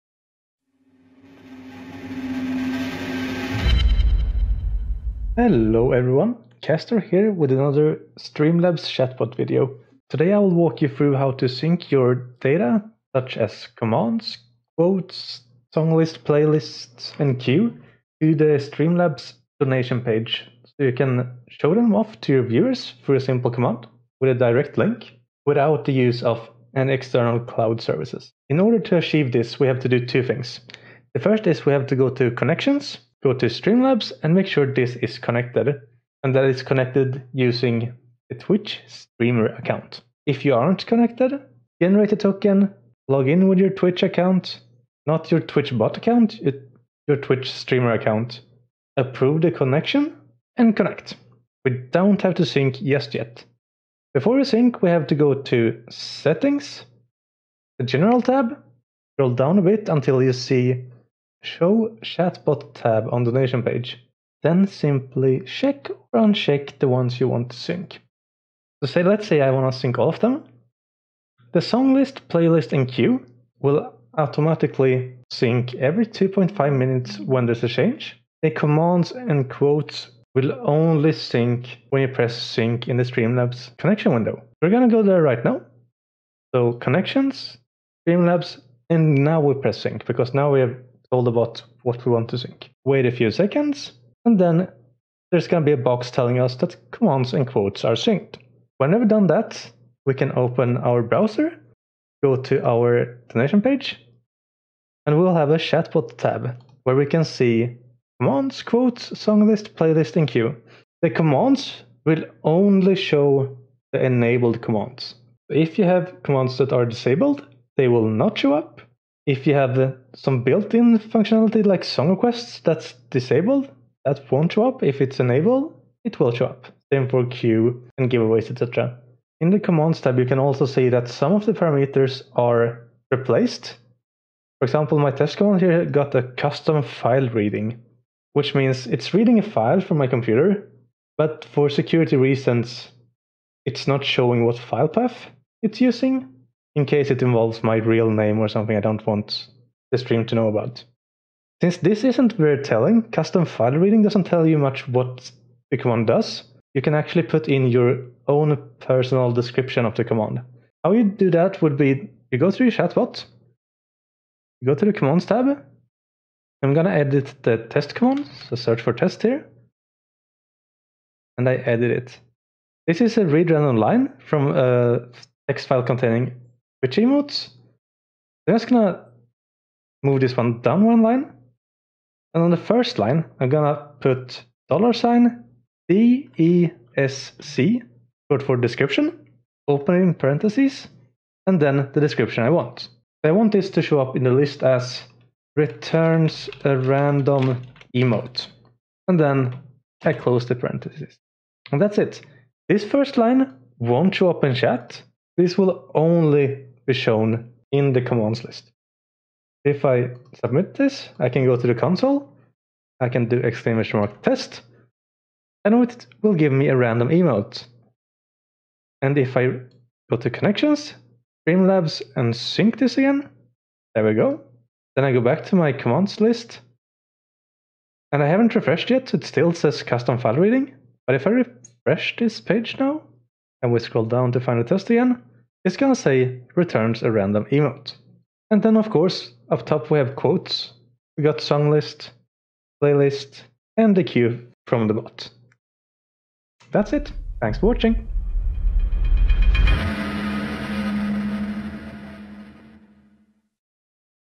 [Streamlabs Chatbot] Share Songlist, Commands etc. the EASY way
How to sync your Songlist, Playlist, Commands, Queue and Quotes to your streamlabs donation page. No need for external cloud services